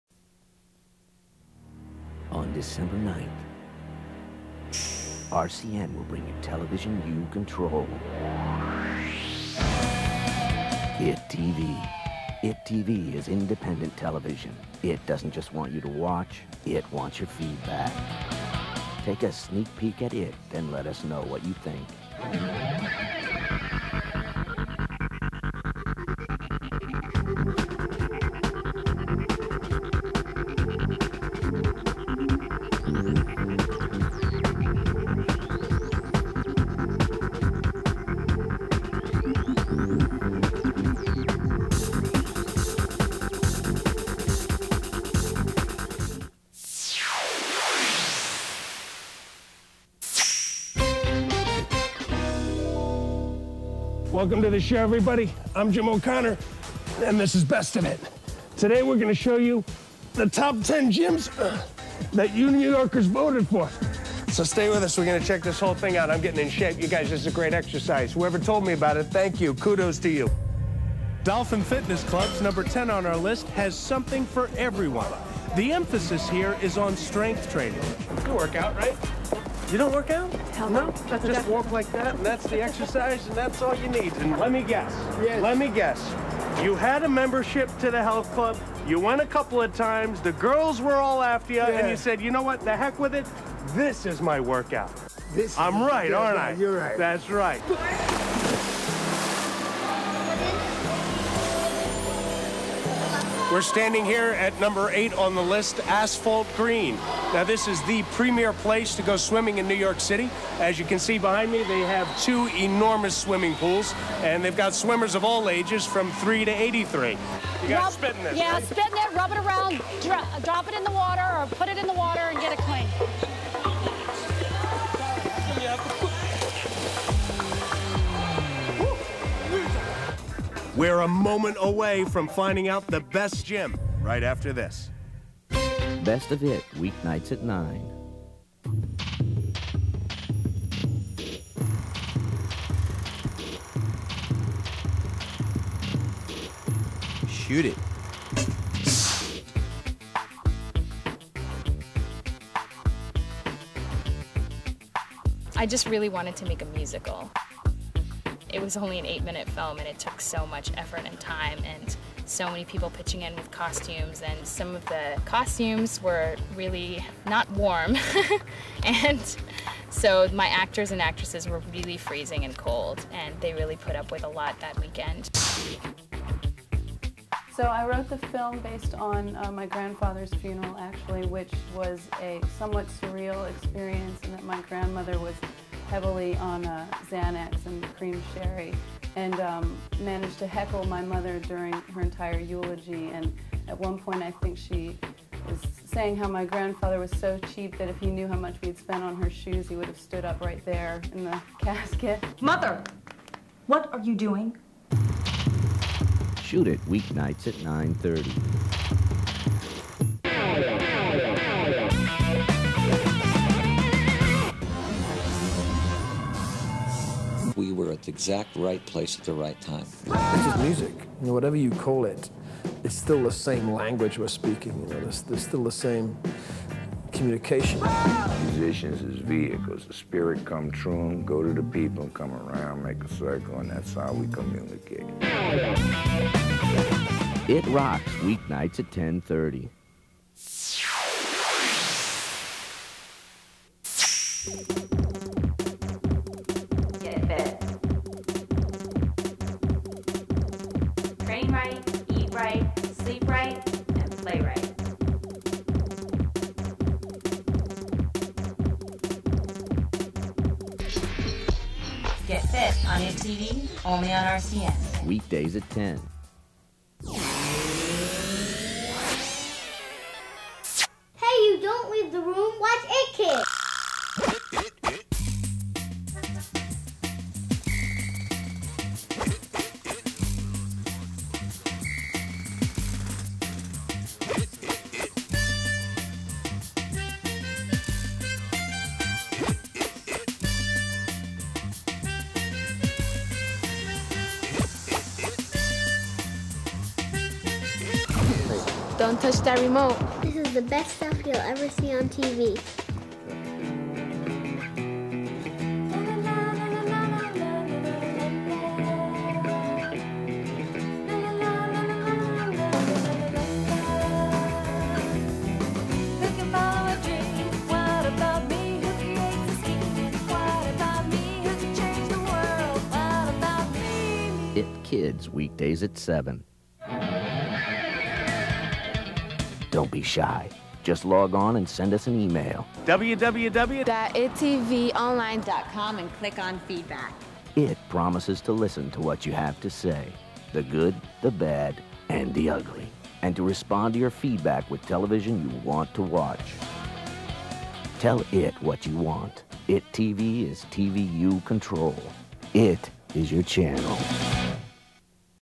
iT TV. Launch video for New York's own TV channel
iT TV EPK.
When you launch a new brand, it always helps to create an EPK (Electronic Press Kit) as part of the launch package. It helps the media and potential investors and advertisers understand more about who you are and what you do, in a bite size package. iT TV Creative Director: Mara Marich